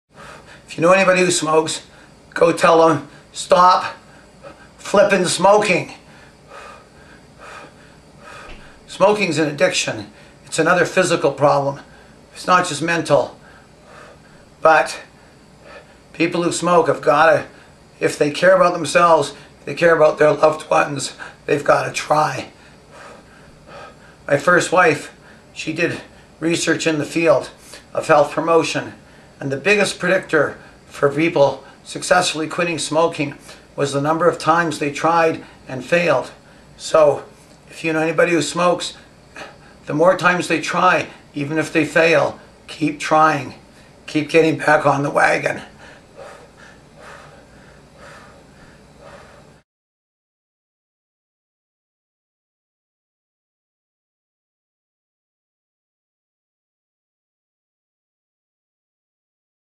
Lung Disease - stop smoking
Smoking caused the diseases for about 8 out of 10 people in my lung disease physiotherapy classes.